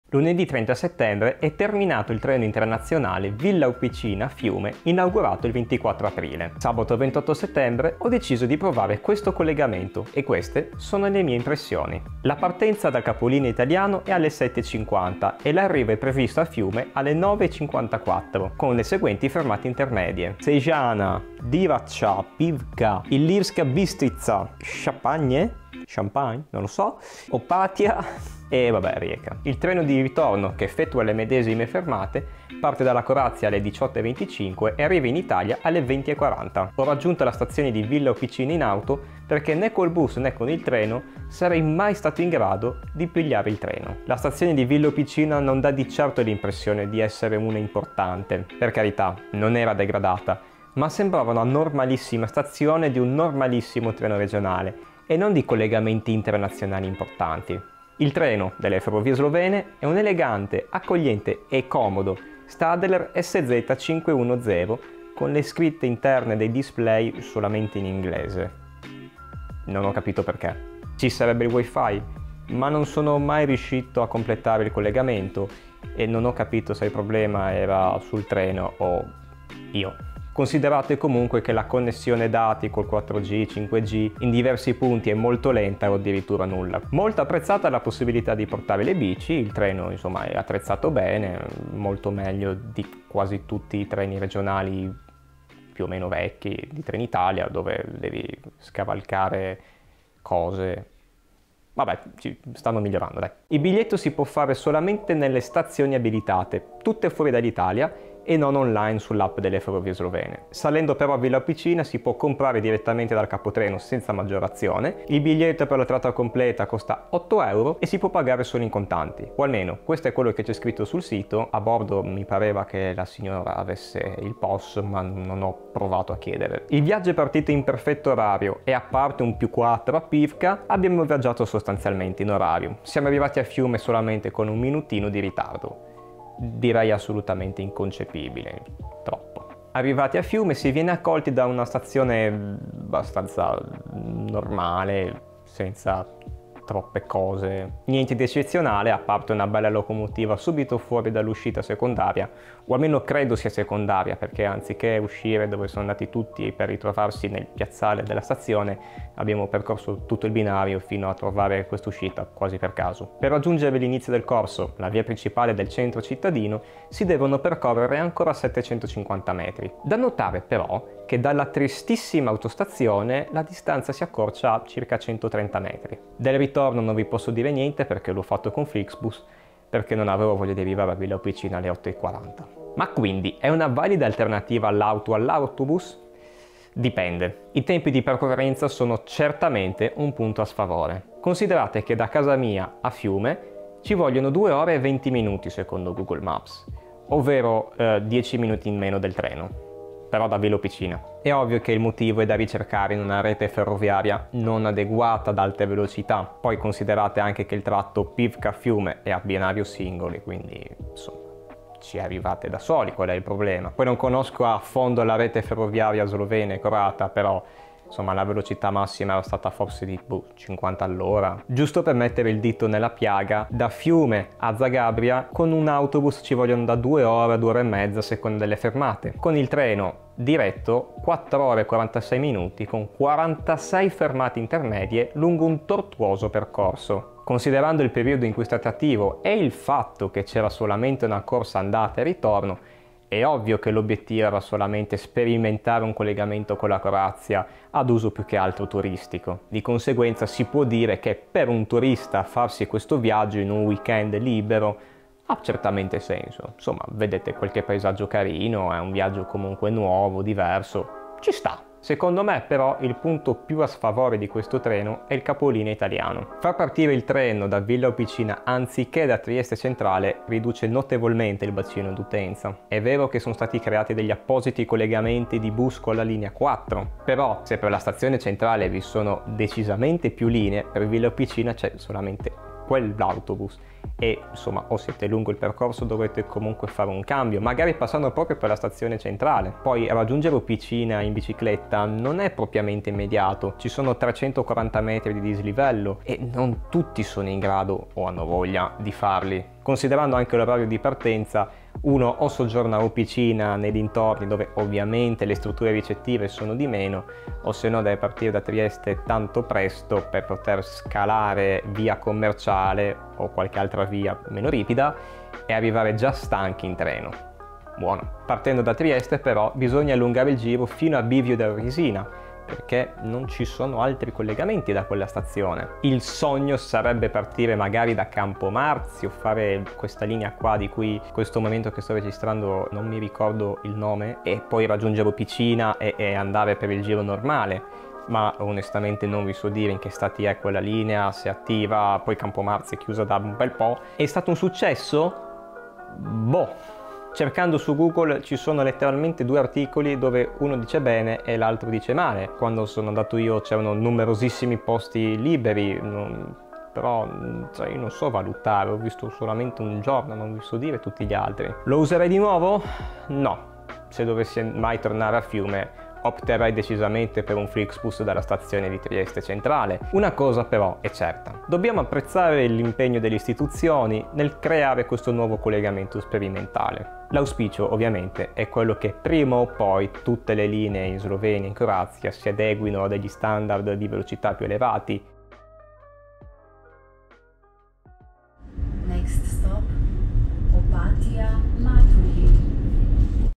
Opinioni sul treno Villa Opicina - Fiume / Rijeka
Un breve ragionamento sul collegamento operato da aprile a settembre tra l'Italia, la Slovenia e la Croazia, ovvero il treno Villa Opicina-Fiume.
Le opinioni si basano su un mio viaggio fatto due giorni prima del termine dell'esperimento.